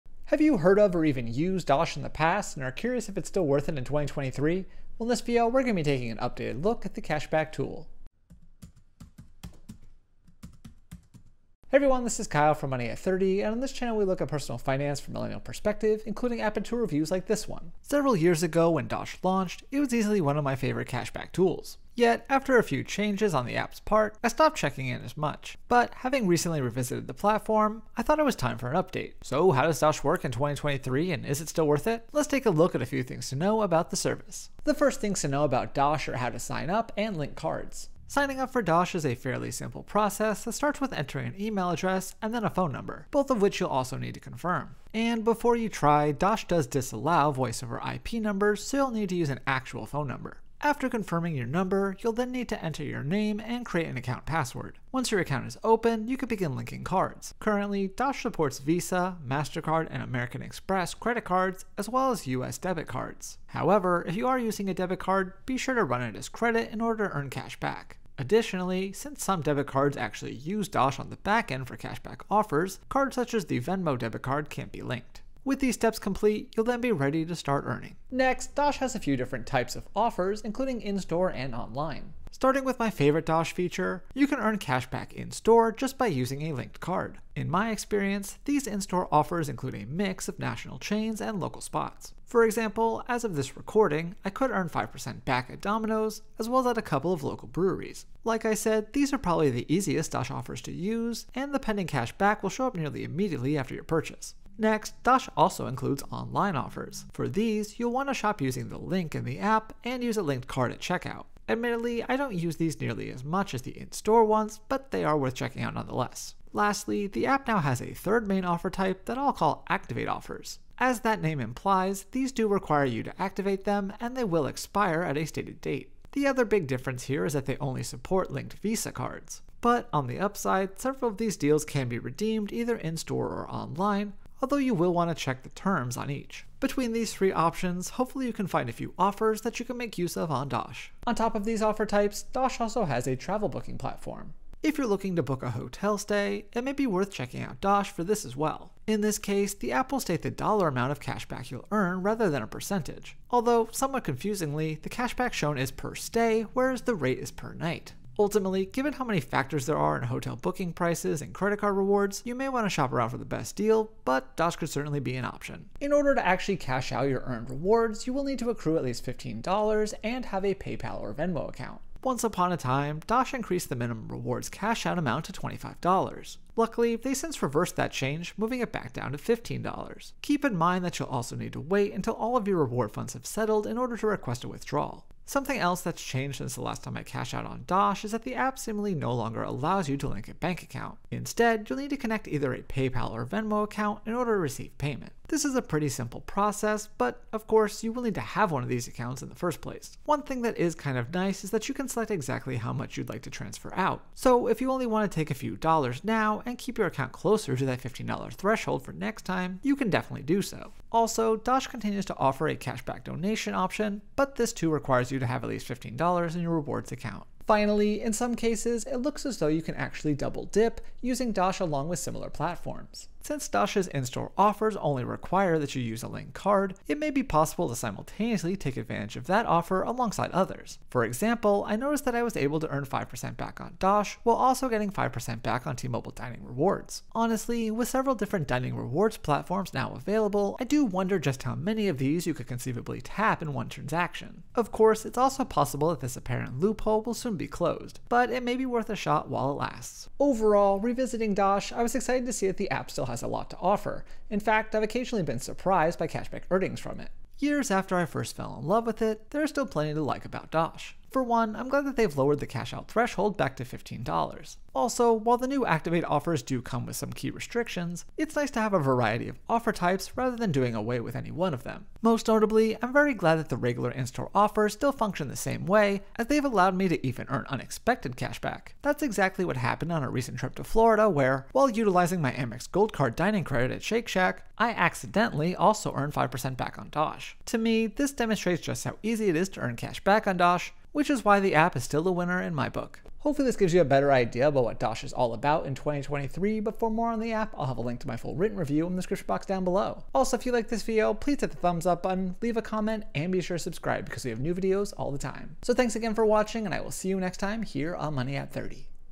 Dosh Cashback App Review - Still Worth It in 2024?
Have you heard of or even used the app Dosh in the past and are wondering if it's still worth it in 2024? Let's take an updated look at the cashback tool.

Several years ago when Dosh launched, it was easily one of my favorite cashback tools. Yet, after a few changes on the app’s part, I stopped checking in as much. But, having recently revisited the platform, I thought it was time for an update. So how does Dosh work in 2023 and is it still worth it? Let’s take a look at a few things to know about the service.

The first things to know about Dosh are how to sign up and link cards

And, before you try, Dosh does disallow VoIP numbers so you’ll need to use an actual phone number. After confirming your number, you’ll then need to enter your name and create an account password.

Once your account is open, you can begin linking cards. Currently, Dosh supports Visa, MasterCard, and American Express credit cards as well as U.S. debit cards. However, if you are using a debit card, be sure to run it as credit in order to earn cashback.

With these steps complete, you’ll then be ready to start earning.

Next, Dosh now has a few different types of offers, including in-store and online

Starting with my favorite Dosh feature, you can earn cashback in-store just by using your linked card. In my experience, these in-store offers include a mix of national chains and local spots. For example, as of this recording, I could earn 5% back at Dominos as well as at a couple of local breweries. Like I said, these are probably the easiest Dosh offers to use and the pending cashback will show up nearly immediately after your purchase.

Next, Dosh also includes online offers. For these, you’ll want to shop using the link in the app and use a linked card at checkout. Admittedly, I don’t use these nearly as much as the in-store ones, but they are worth checking out nonetheless.

Lastly, the app now has a third main offer type I’ll call activate offers. As that name implies, these do require you to activate them and they will expire at a stated date. The other big difference here is that they only support linked Visa cards. But, on the upside, several of these deals can be redeemed either in-store or online - although you will want to check the terms on each.

Between these three options, hopefully you can find a few offers you can make use of on Dosh.

On top of those offer types, Dosh also has a travel booking platform

If you’re looking to book a hotel stay, it may be worth checking out Dosh for this as well. In this case, the app will state the dollar amount of cashback you’ll earn rather than the percentage. Although, somewhat confusing, the cashback shown is per stay whereas the rate is per night.

Ultimately, given how many factors there are in hotel booking prices and credit card rewards, you may want to shop around for the best deal - but Dosh could certainly be an option.

Once upon a time, Dosh increased the minimum rewards cashout amount to $25. Luckily, they’ve since reversed that change, moving it back down to $15. Keep in mind that you’ll also need to wait until all of your reward funds have settled in order to request a withdrawal.

Something else that’s changed since the last time I cashed out on Dosh is that the app seemingly no longer allows you to link a bank account. This is a pretty simple process, but of course you will need to have one of these accounts in the first place.

One thing that is kind of nice is that you select exactly how much you’d like to transfer out. So, if you only want to take a few dollars now and keep your account closer to that $15 threshold for next time, you can definitely do so.

Also, Dosh continues to offer a cashback donation option, but this too requires you to have at least $15 in your rewards account.